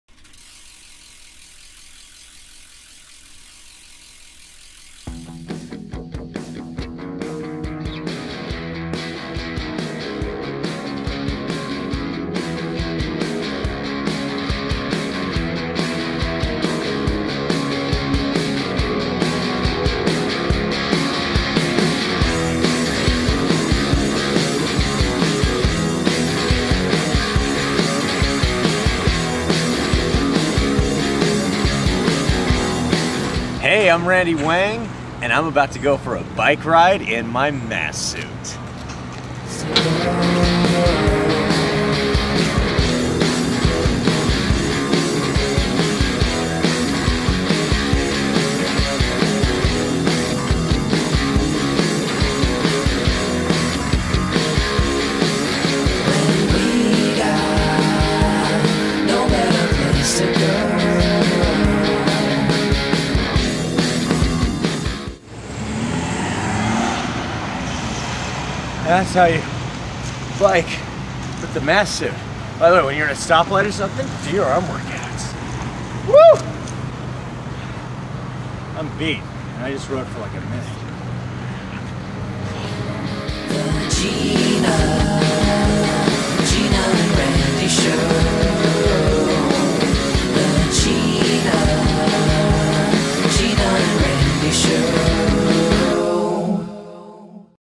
Mass Suit Series - Riding a Bike
Randy Wang demonstrates how to use the Mass Suit on his bicycle.